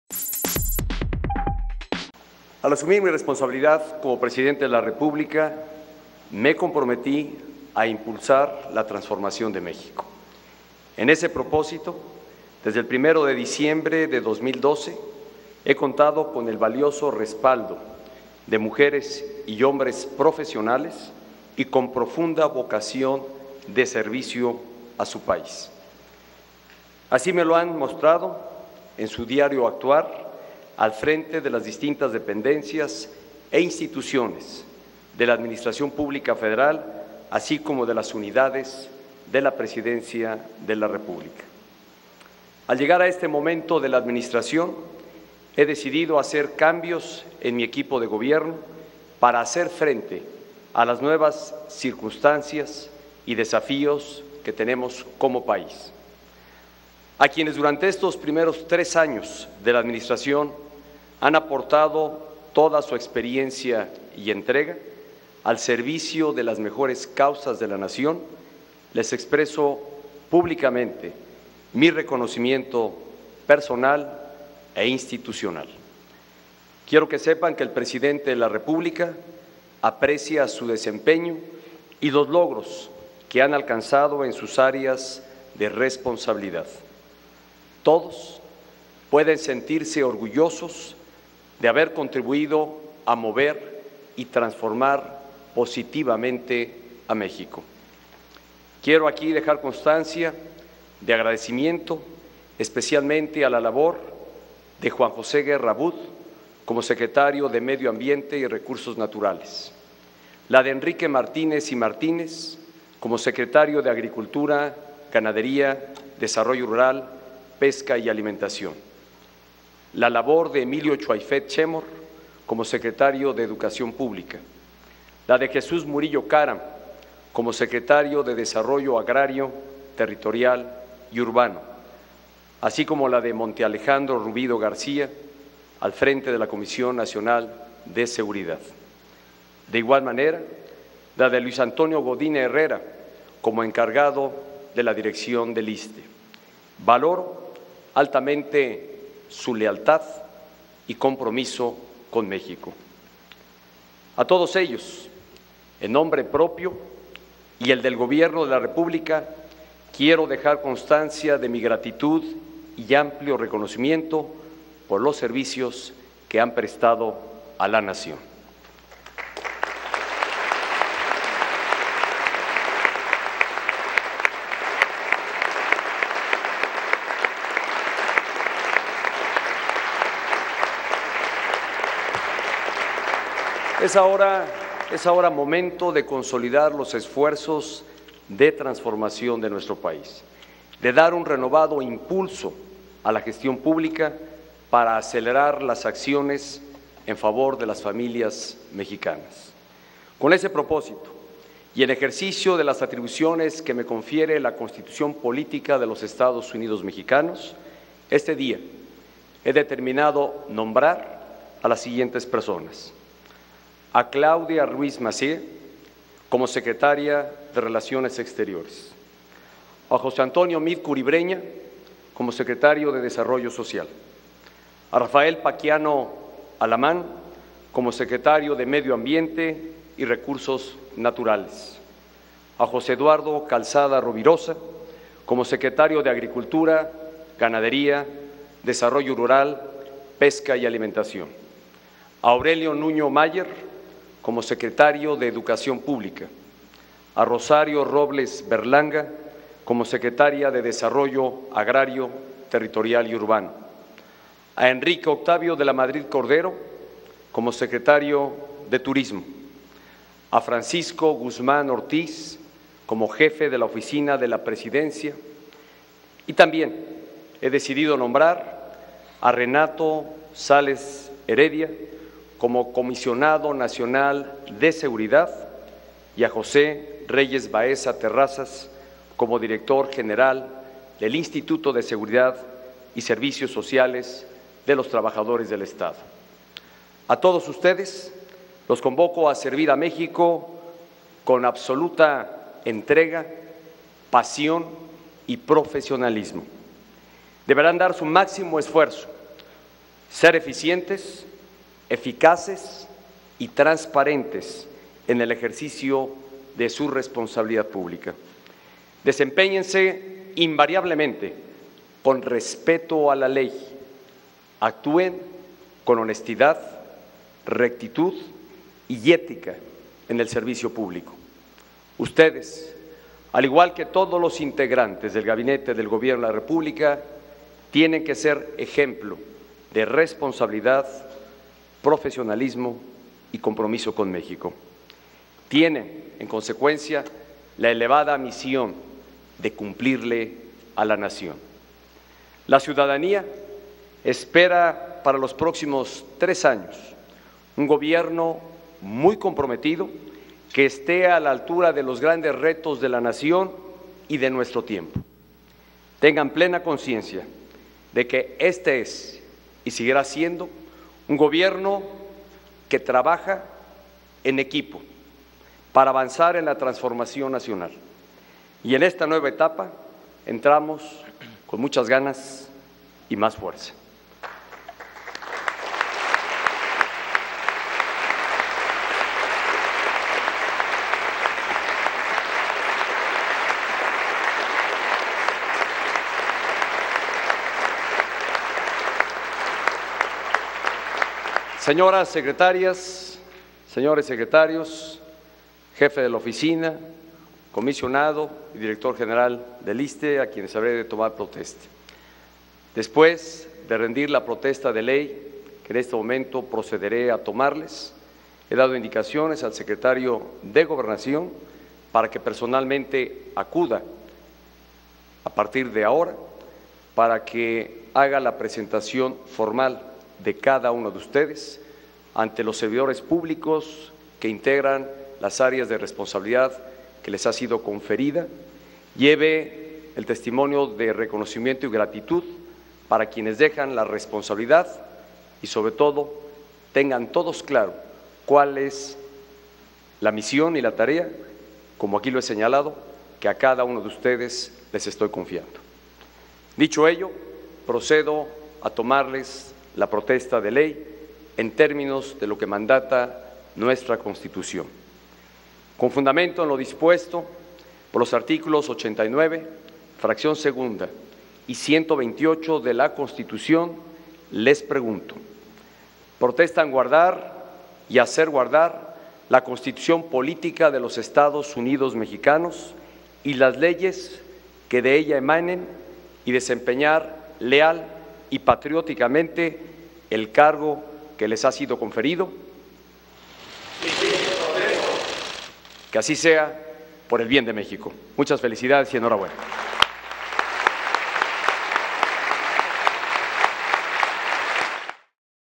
Anuncia Presidente Peña Nieto 10 cambios en su gabinete
Aurelio Nuño Mayer a la SEP; Francisco Guzmán Ortíz, a la Oficina de la Presidencia de la República, Rosario Robles a Sedatu; Claudia Ruiz Massieu a SRE